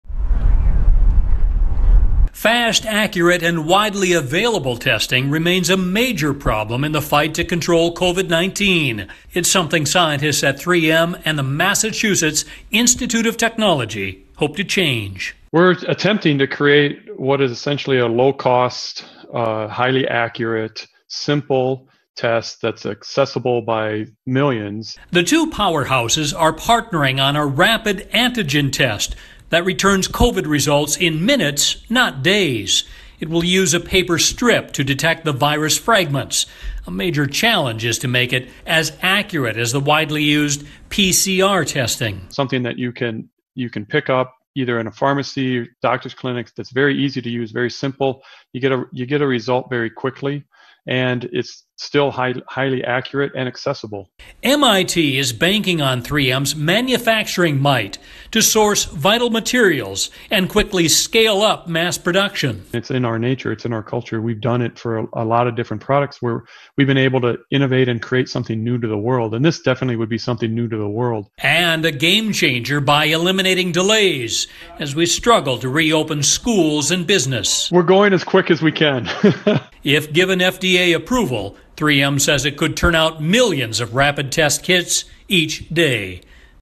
3M partners with MIT for a COVID-19 test that shows results in minutes.
3M partners with MIT for a COVID-19 test that shows results in minutes.